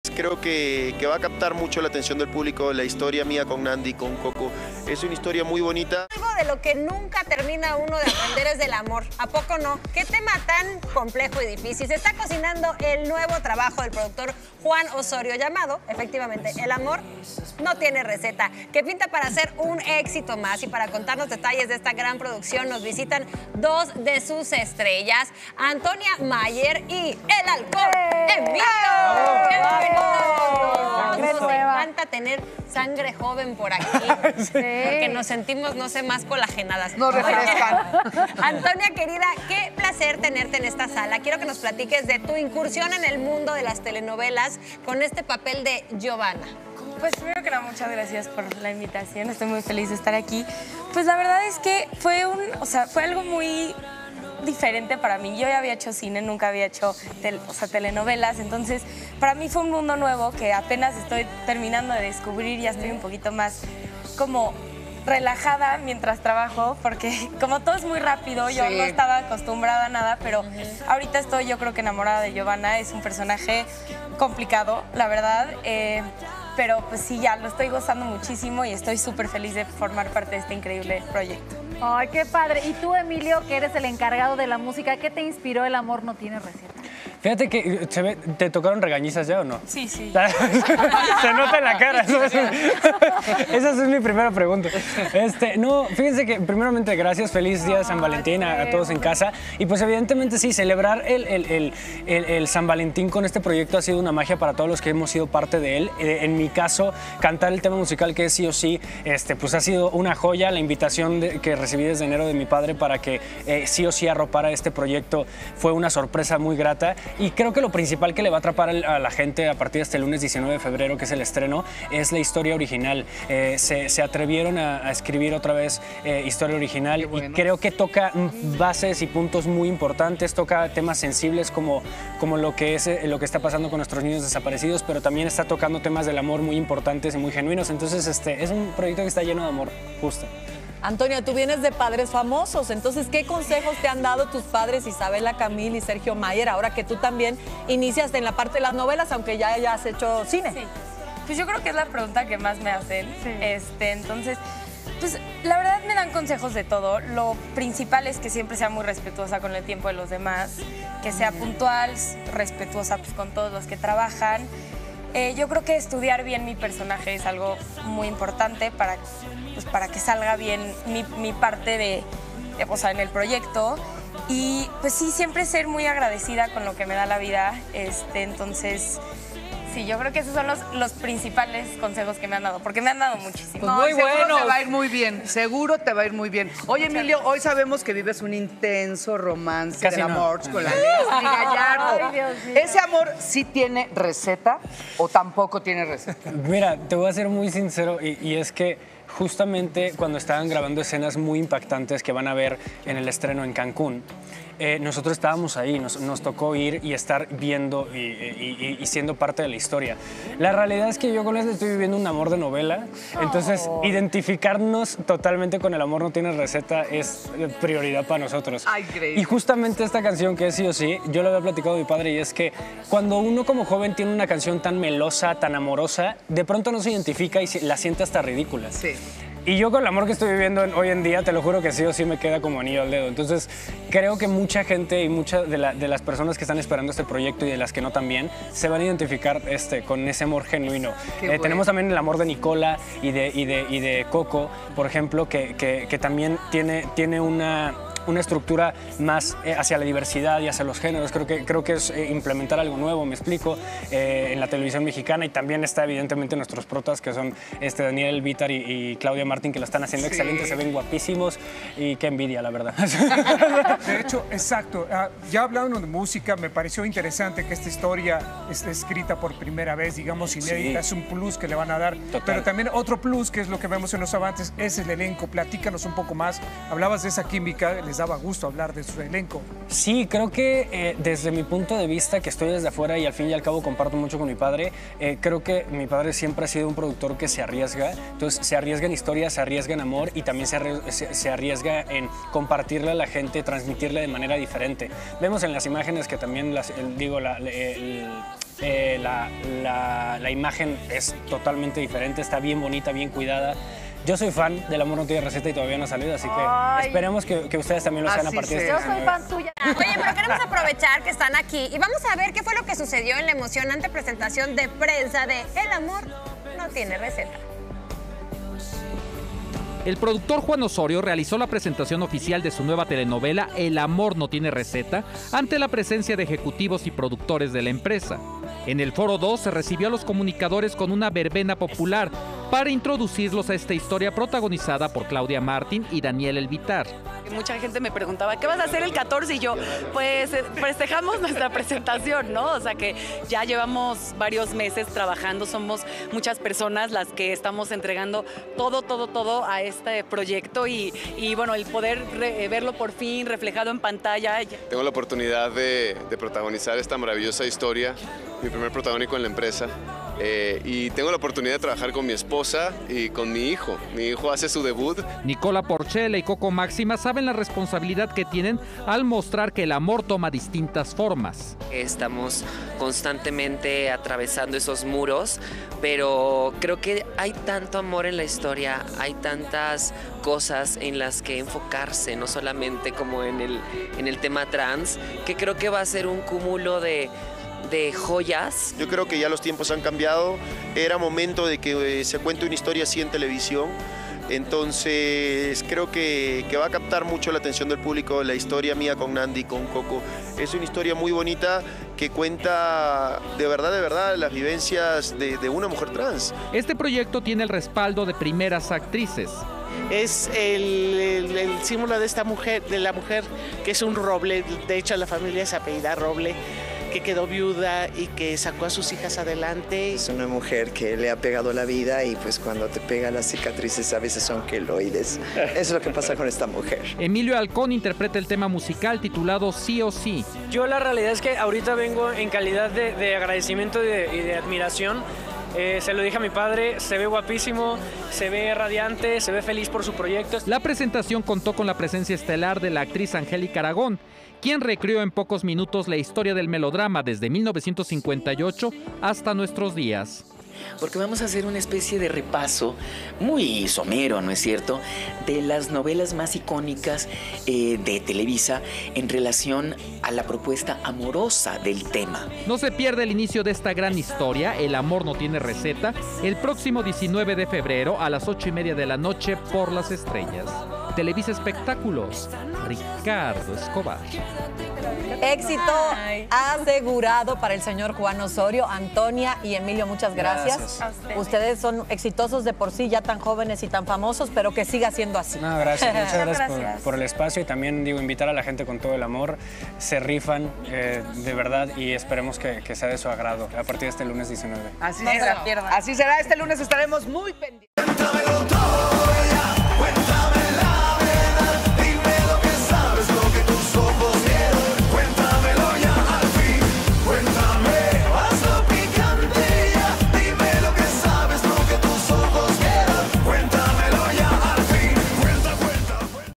Nicola Porcella asegura que México está listo para una novela como 'El amor no tiene receta' | CYA
Emilio Halcón y Antonia Mayer nos hablaron sobre la esencia de este melodrama y todas las sorpresas que esconde la nueva producción de Juan Osorio. Disfruta de las actuaciones de Claudia Martín, Daniel Elbittar, Nicola Porcella y Coco Máxima en esta telenovela a partir del 19 de febrero a las 8:30 por Las Estrellas.

Las Estrellas te ofrece el mejor contenido de telenovelas, notas exclusivas, programas, avances y noticias de tus artistas favoritos.